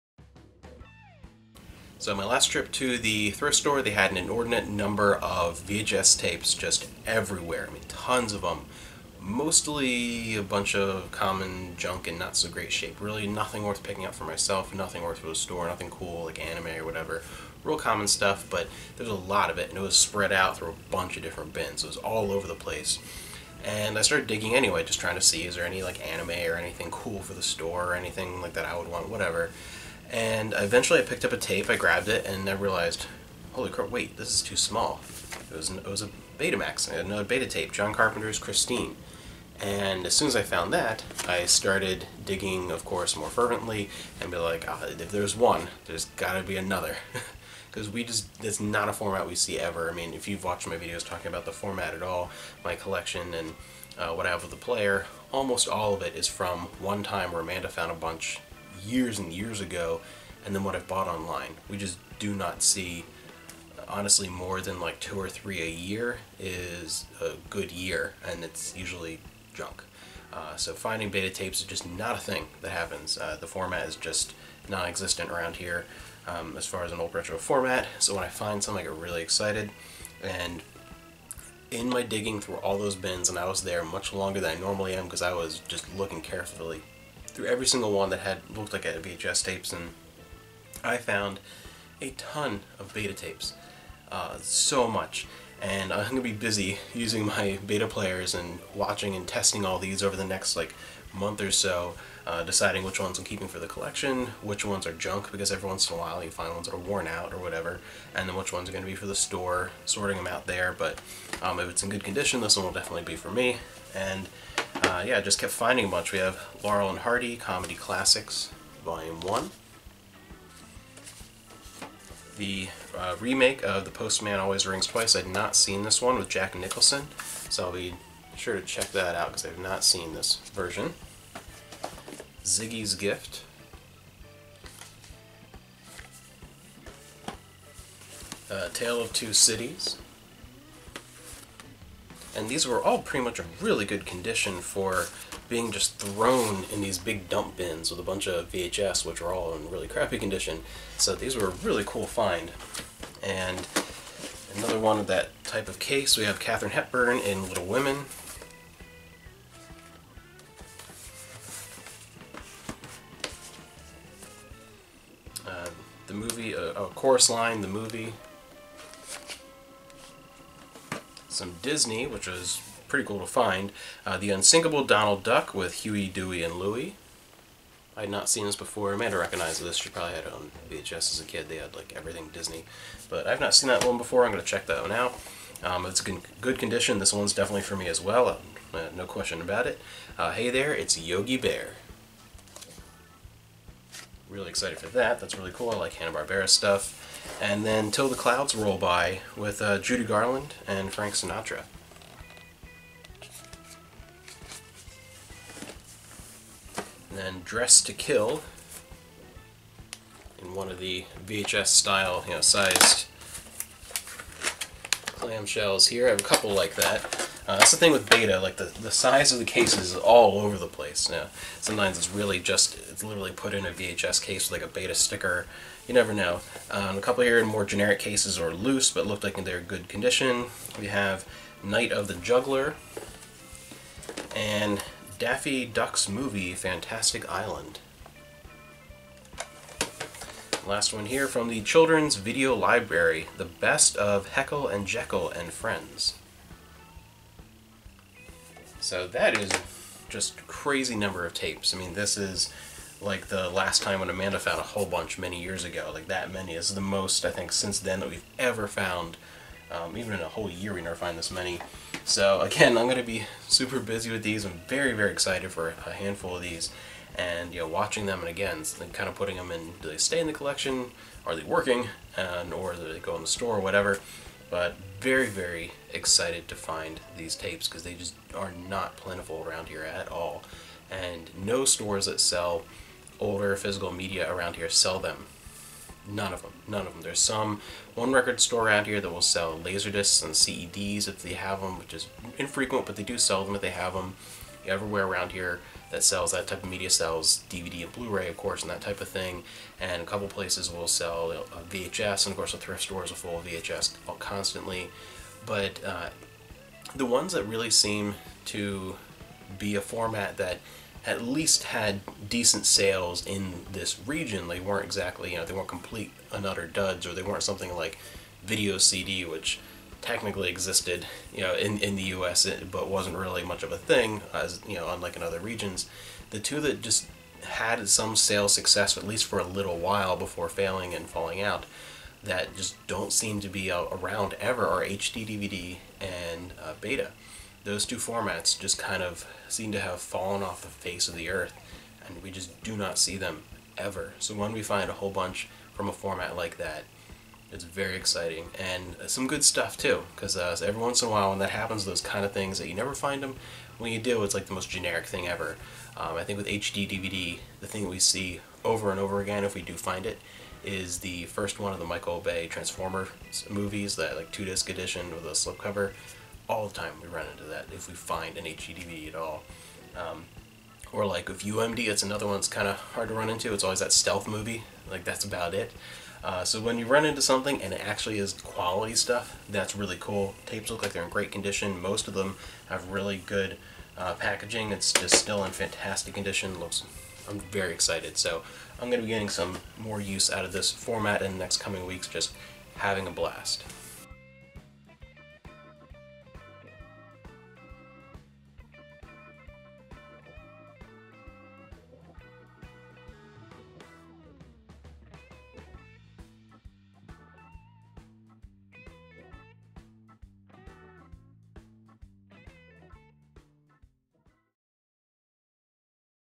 Surprise Beta tape collection finds!
During my latest thrift store trip, I stumbled upon a large number of Beta tapes! We don't find Betamax very often, so this was awesome!